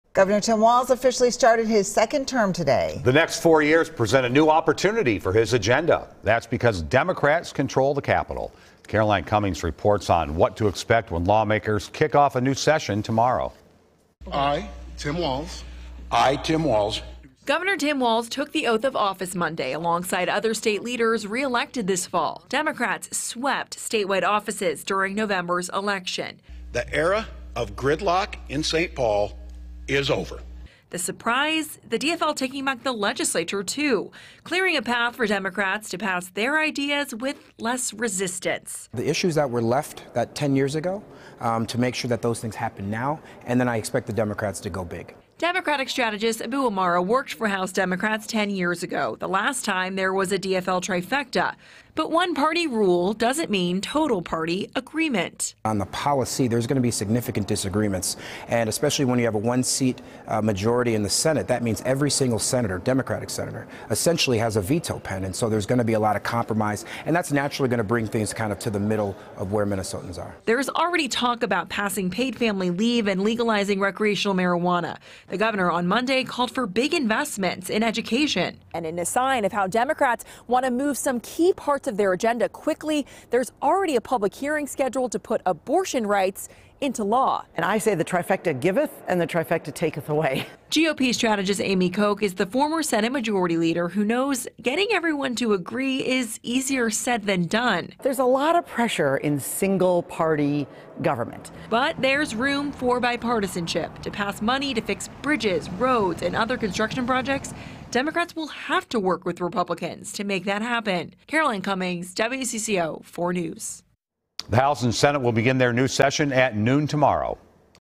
What to expect when Minnesota lawmakers start new session
The next four years present a new opportunity for Gov. Tim Walz’s agenda, because Democrats now have total control at the Capitol.